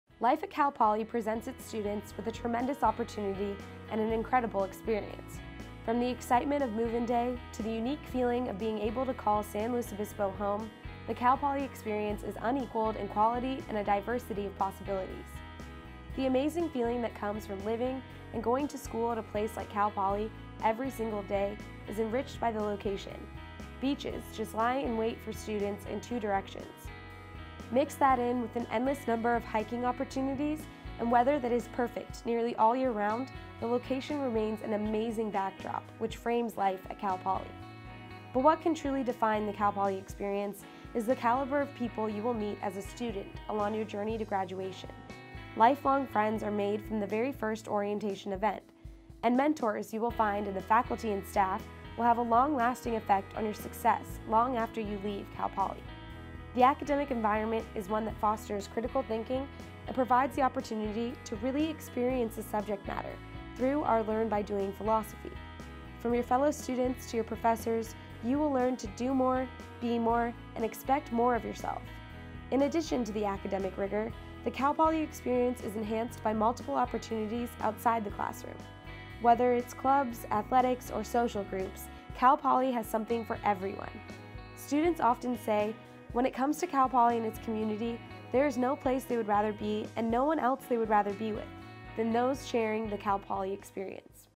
Throw Back Thursday - Life at Cal Poly Commercial
In this weeks Throwback Thursday video we throw it back to our Cal Poly Life at Cal Poly commercial. This video was sent to students highlighting the Cal Poly campus and experience at Cal Poly.